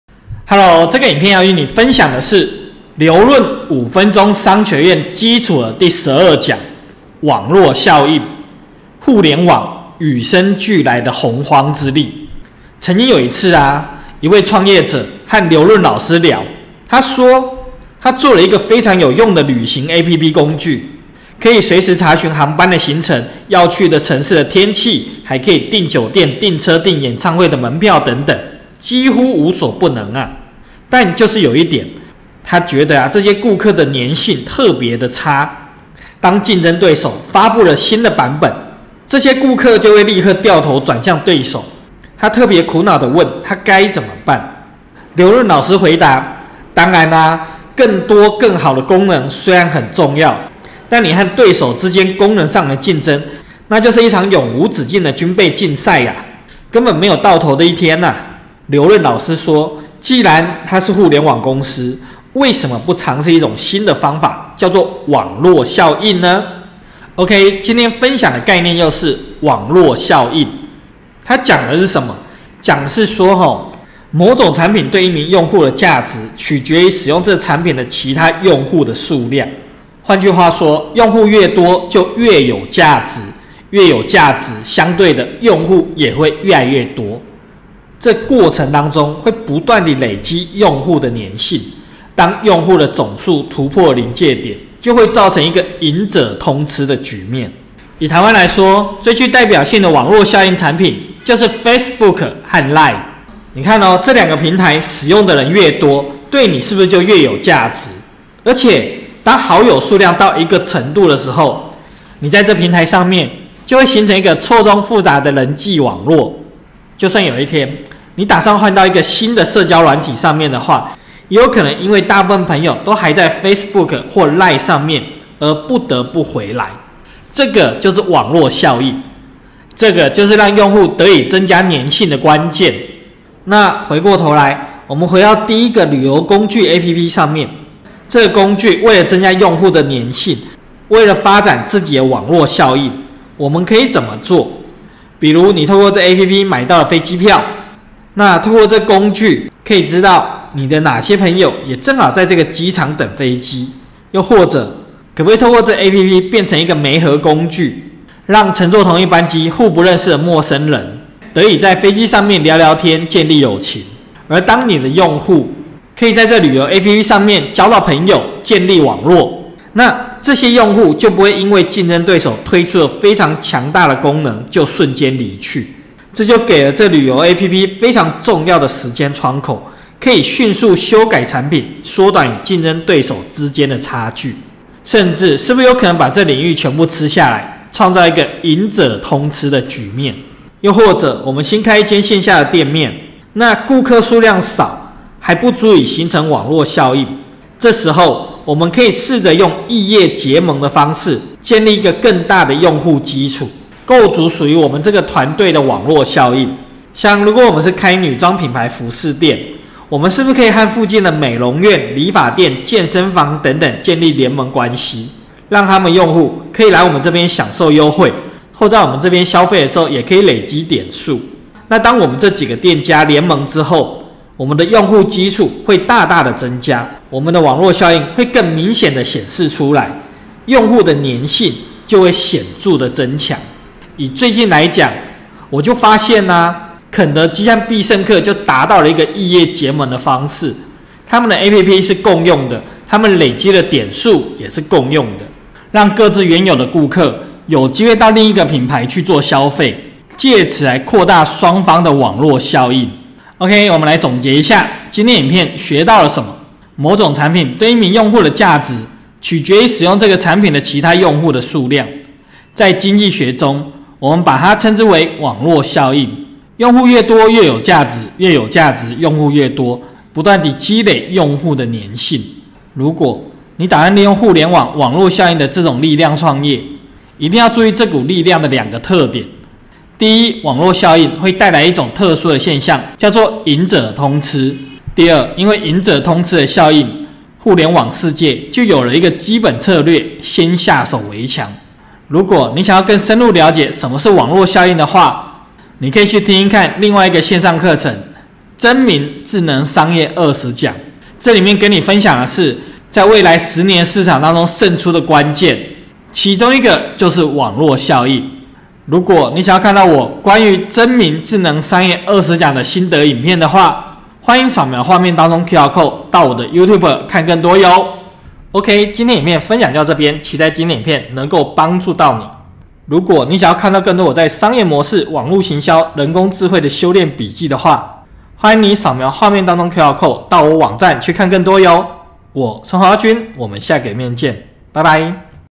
互聯網與生俱來的洪荒之力｜劉潤《5分鐘商學院・基礎》-12-網絡效應
某種產品對一名用戶的價值，取決於使用這個產品使用這個產品的其他用戶數量，在經濟學中，我們把它稱之為：網絡效應。
用戶越多，越有價值；越有價值，用戶越多；不斷地積累用戶的黏性。

如果你打算利用互聯網“網絡效應”的這種神力創業，一定要注意這股力量的兩個特點：
第一，網絡效應會帶來一種特殊的現象，叫：贏家通吃。
第二，因為贏家通吃的效應，互聯網世界就有了一個基本策略：先下手為強。